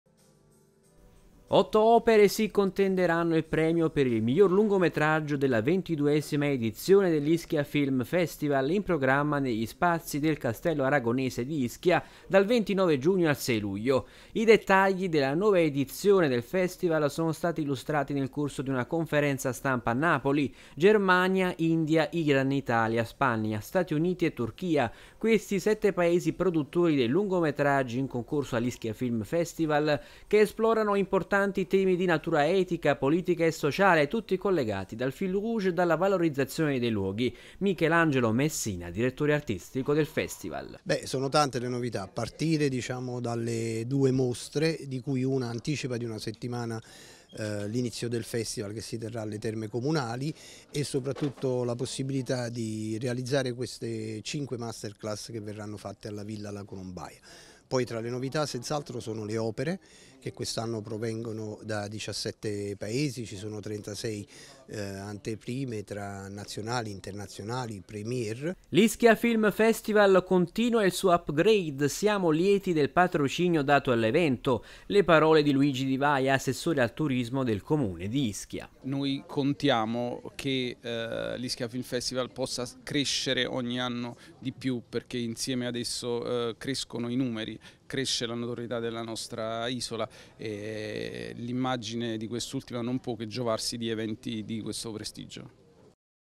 ISCHIA FILM FESTIVAL AL VIA IL 29 GIUGNO, PRESENTATA AL MOLO BEVERELLO L’EDIZIONE 2024
Otto opere si contenderanno il premio per il miglior lungometraggio della 22^ edizione dell’Ischia Film Festival, in programma negli spazi del Castello Aragonese di Ischia dal 29 giugno al 6 luglio 2024, con il sostegno della Direzione Generale Cinema e Audiovisivo del MIC e della Regione Campania. I dettagli della nuova edizione del festival sono stati illustrati nel corso di una conferenza stampa a Napoli.

Germania, India, Iran, Italia, Spagna, Stati Uniti e Turchia: questi i sette paesi produttori dei lungometraggi in concorso all’ischia film festival che esplorano importanti temi di natura etica, politica e sociale, tutti collegati dal fil rouge della valorizzazione dei luoghi.